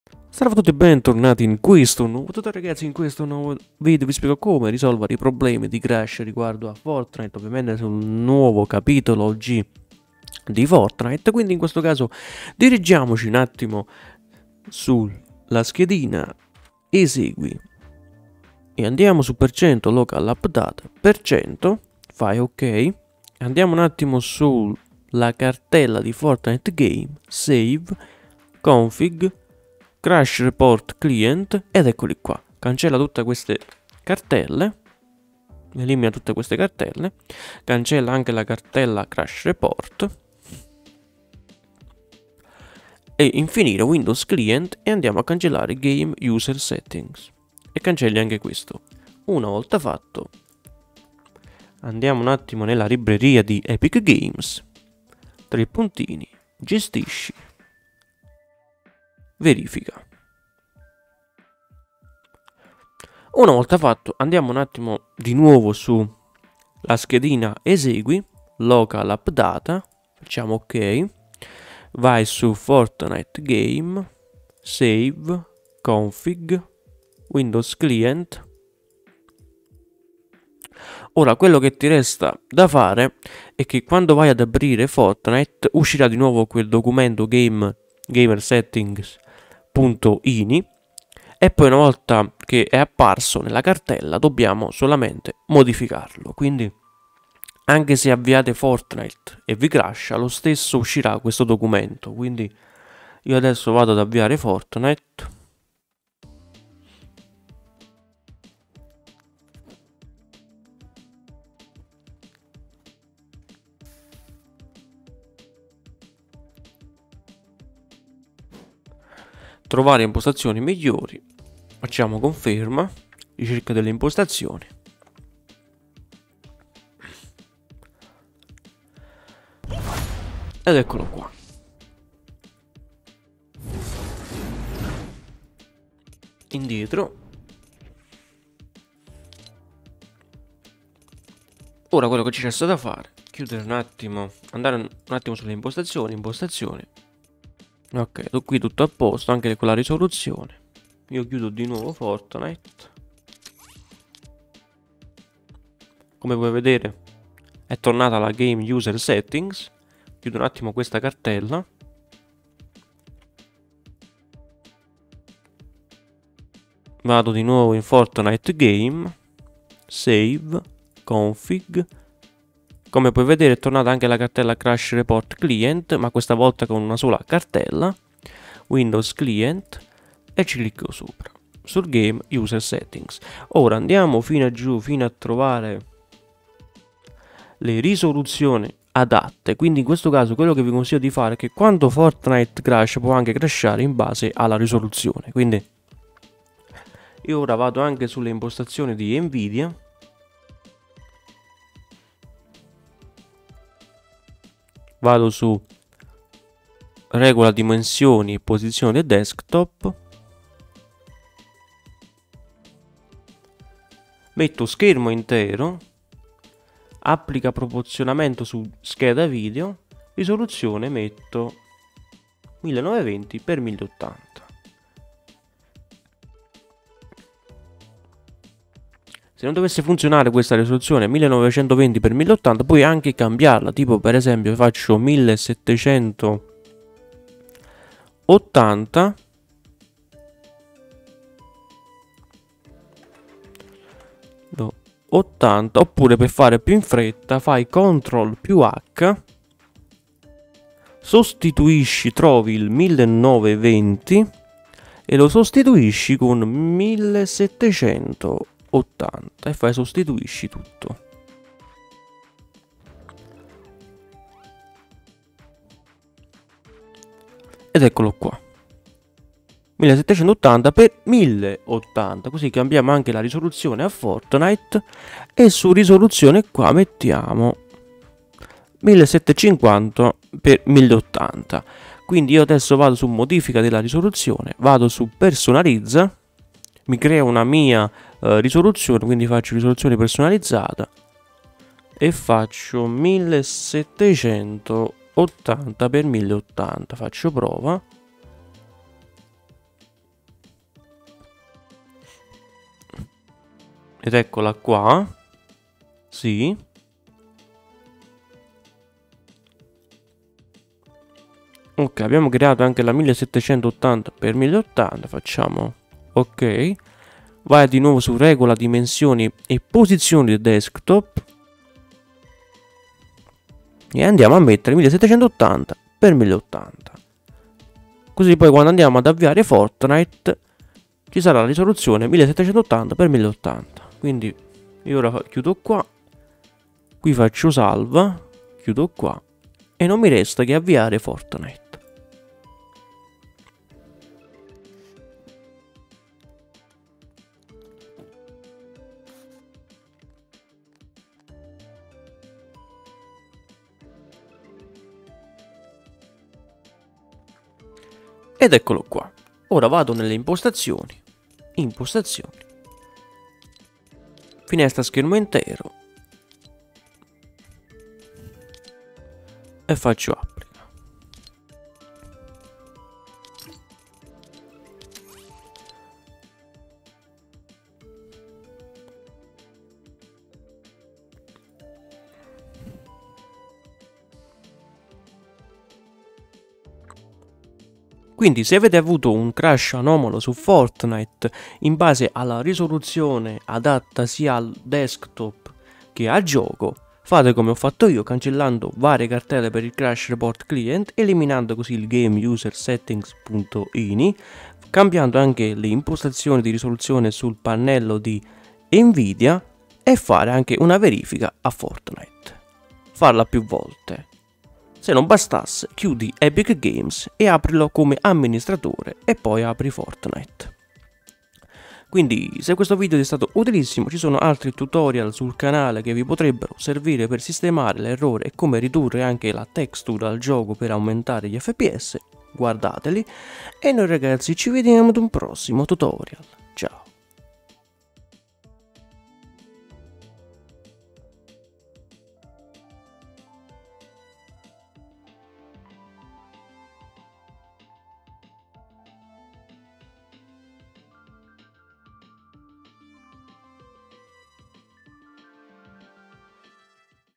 FORTNITE CRASHA SEMPRE | PROBLEMI DI RISOLUZIONI E CRASH REPORT | COME RISOLVERE - TUTORIAL
Fortnite crasha e si blocca senza nessun errore, ecco come risolvere il problema! ;)

MIO REFCODE:563685ph1

PC:
MOBO: B450 GAMING PLUS
CPU: R5 3600
GPU: 1660 SUPER
RAM: 16 GB
CASE: 4000D AIRFLOW CASE CORSAIR

VIDEO TUTORIAL
               GAMES---TECH---MICROSOFT